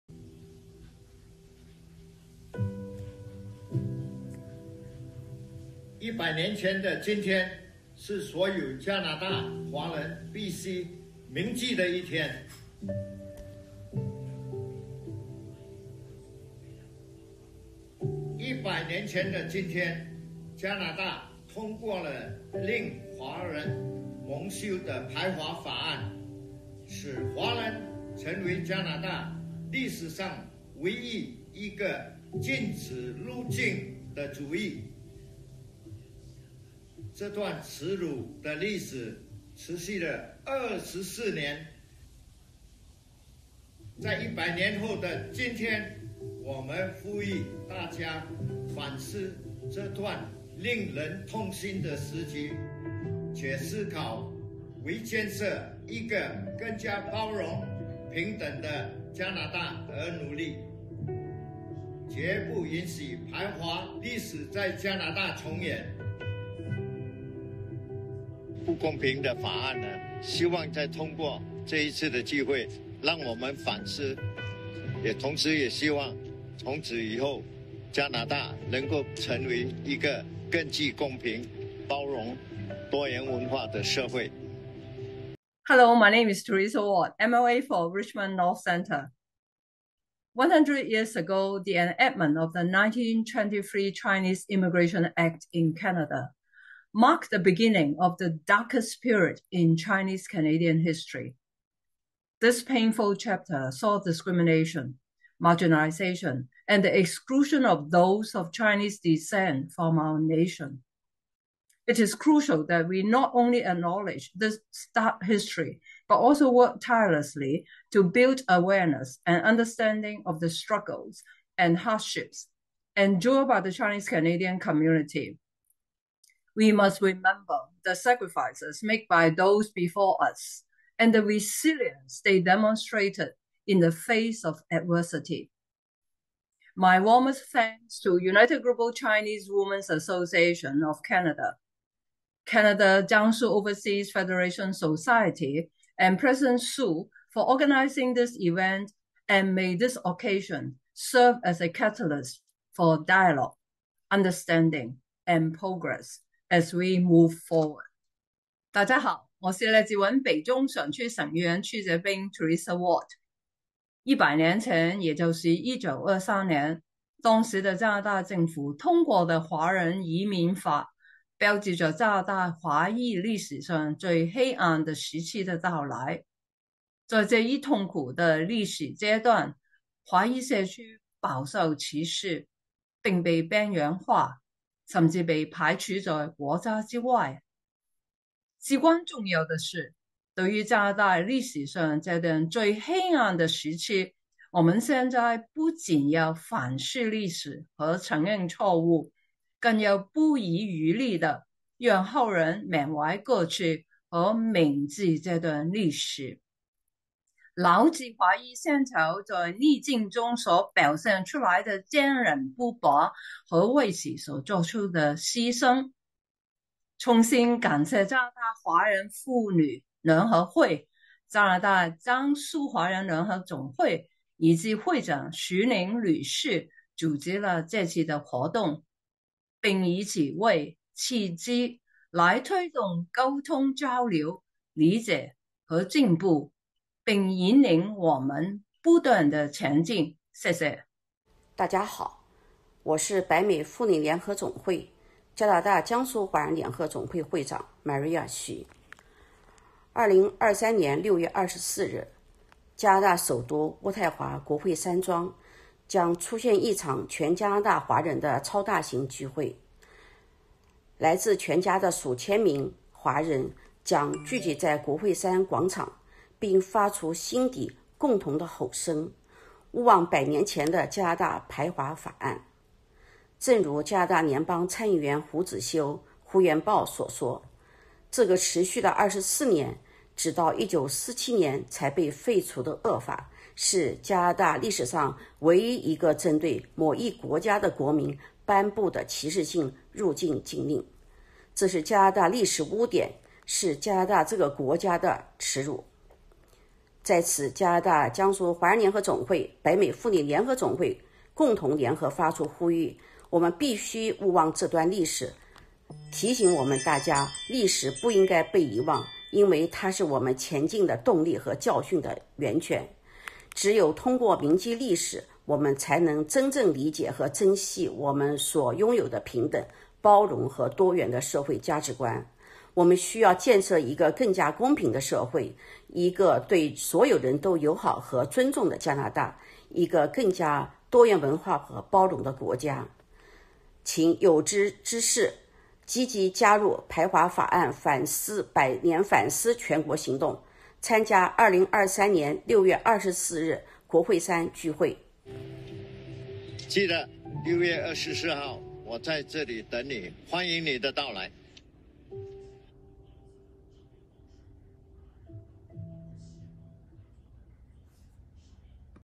加拿大江苏华人联合总会和北美妇女联合总会呼吁勿忘百年排华法案
在纪念加拿大反排华法案100周年的同时，让我们共同承诺，永远不再重蹈历史的覆辙。让我们铭记过去的痛苦，但不被困于其中。让我们以包容和友爱为指引，建设一个更加公正、平等和多元的社会。

在这个值得纪念的时刻，让我们向所有为反排华法案而奋斗的人们致敬。他们是勇敢的先驱者，为了正义和平等，付出了巨大的努力和牺牲。我们应该铭记他们的奋斗和牺牲，将他们的精神传承下去。

加拿大江苏同乡联谊总会和北美妇女联合总会的呼吁提醒我们，我们有责任铭记历史，反思过去的错误，并以此为基础，为一个更加公正和包容的未来而奋斗。让我们携起手来，共同努力，为我们的社会创造一个更美好的明天。

6月24日，让我们相约去渥太华！我们不见不散！